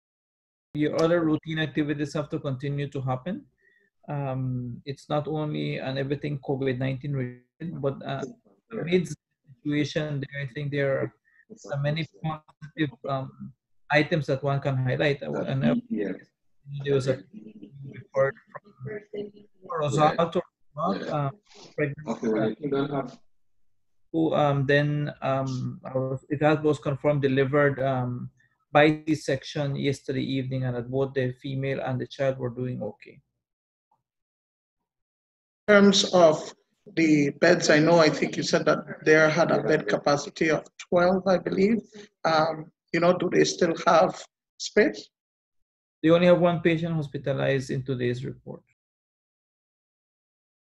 Only 1 COVID-19 Hospitalized at Northern Regional Hospital
With rising COVID numbers in Orange Walk, there is added stress on the Northern Regional Hospital which is the main facility in the north for the hospitalization of critical COVID-19 patients. That aside, the NRH has to treat other patients and also engage in contact tracing. DHS Marvin Manzanero says that fortunately at this time there is only one COVID-19 patient hospitalized at the NRH.